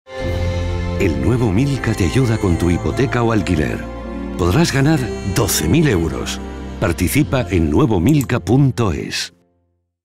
El NuevoMilka te ayuda con tu hipoteca o alquiler
Sabemos que son tiempos difíciles y el NuevoMilka puede ayudarte con tu hipoteca o alquiler 🍫💜🐮 ¡Participa en nuevo.milka.es!